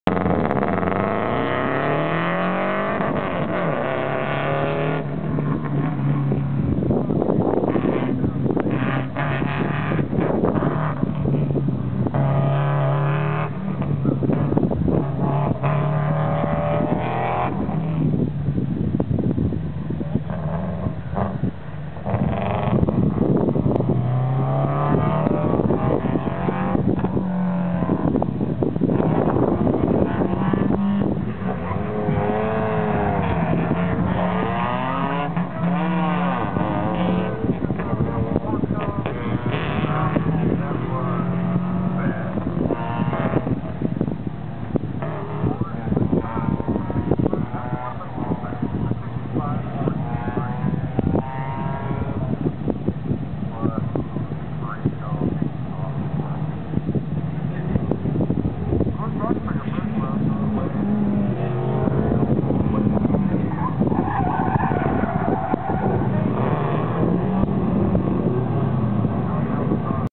Mid State Airport Autocross Subaru RS
Subaru RS rallycross car at the Mid State Airport autocross. Central PA SCCA